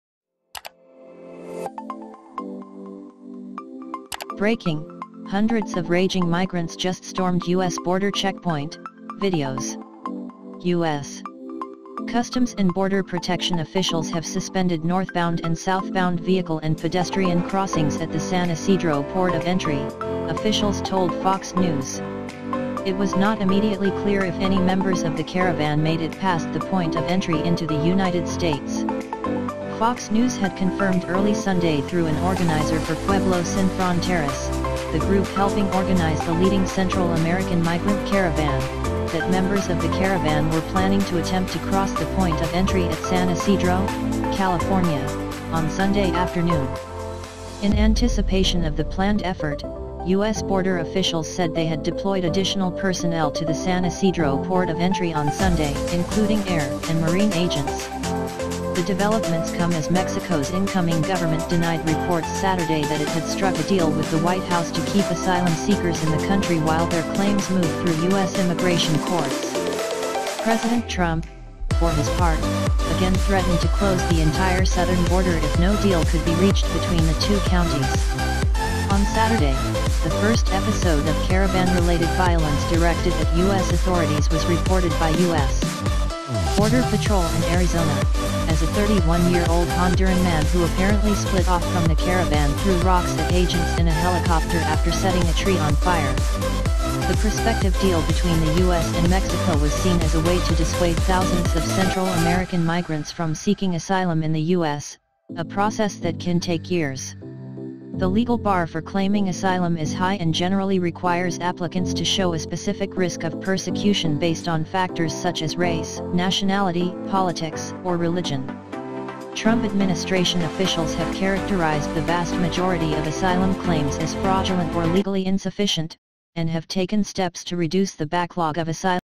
BREAKING: Hundreds Of Raging Migrants Just Stormed US Border Checkpoint (VIDEOS) Breaking news
Breaking news Hundreds Of Raging Migrants Just Stormed US Border Checkpoint (VIDEOS)